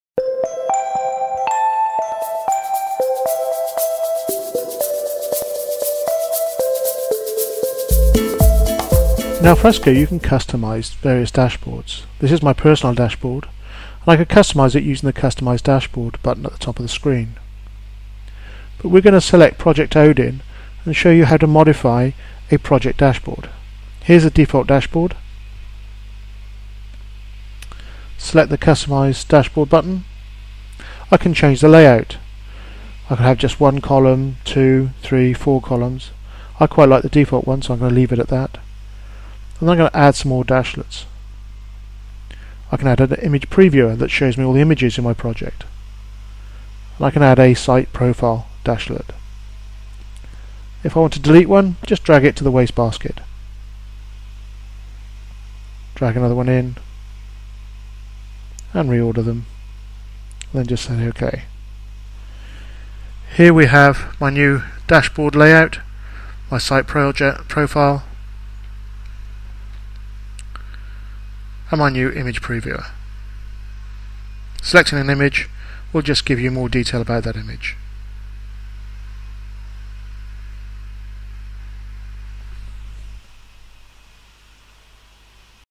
Alfresco: Customise the dashboard
This video shows you how to customise your dashboards in Alfresco in the cloud